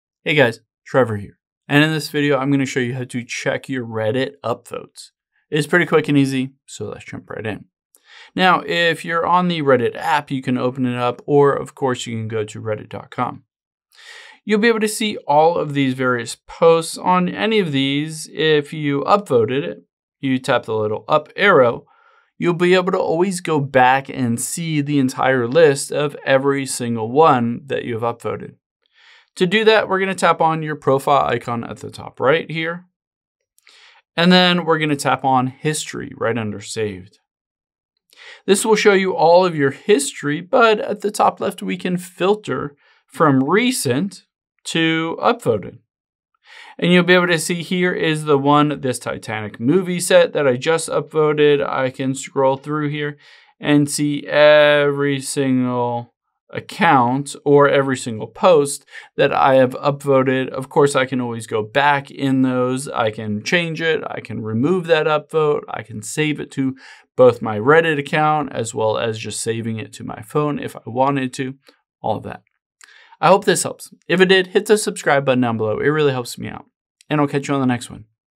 How To Check Reddit Upvotes
Here's how to check your Reddit upvotes if you want to see which past posts you upvoted if you want to change it or just read it again.